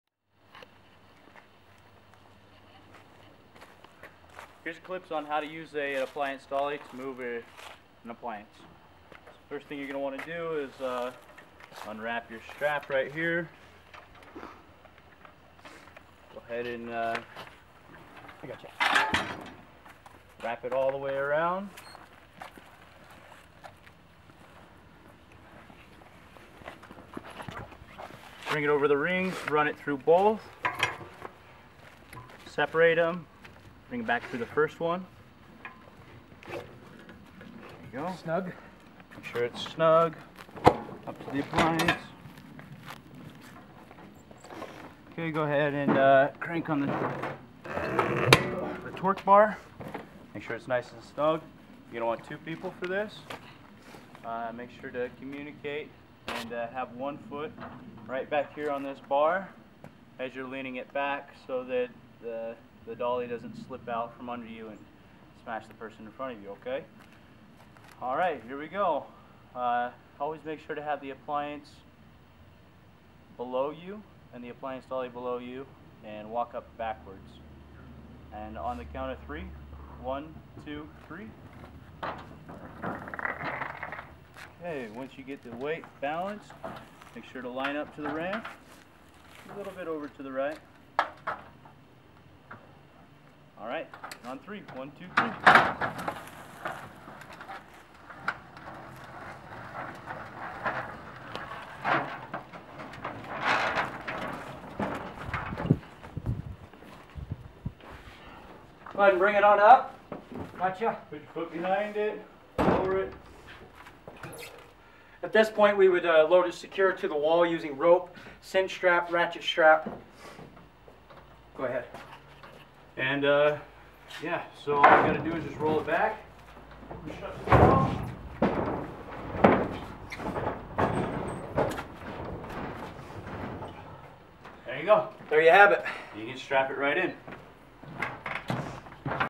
Moving Connections: Appliance Moving
Moving Connections shows how to properly use an appliance dolly to move appliances like freezers and refrigerators.